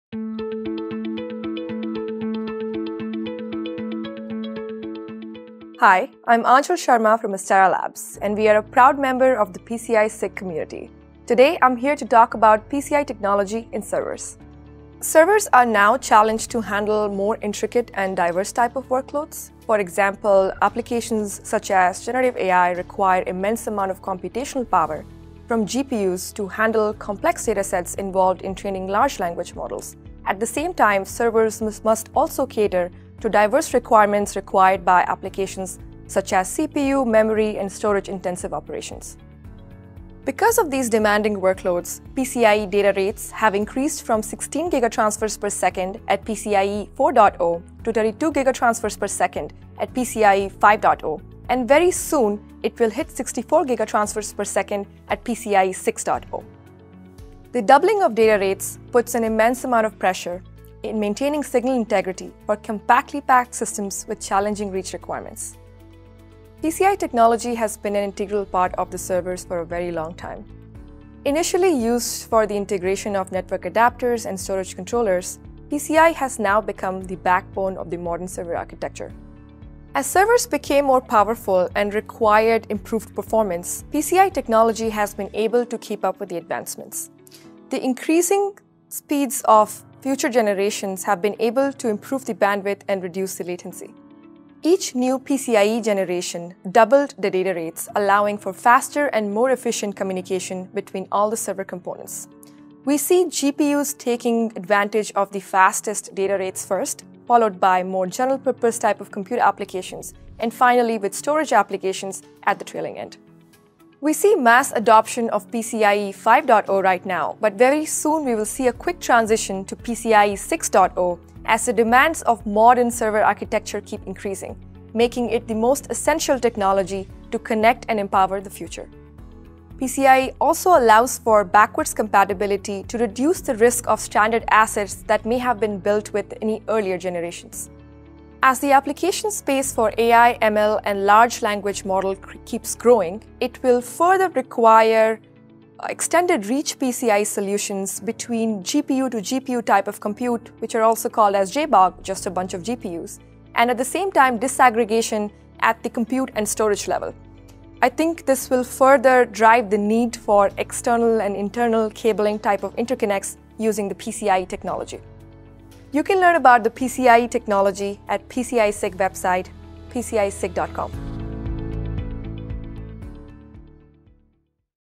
PCIe Technology in Servers
In this video, PCI-SIG Member Aanchal Sharma of Astera Labs discusses the integral role PCI Express technology plays in modern server architectures where high bandwidth and low latency is required.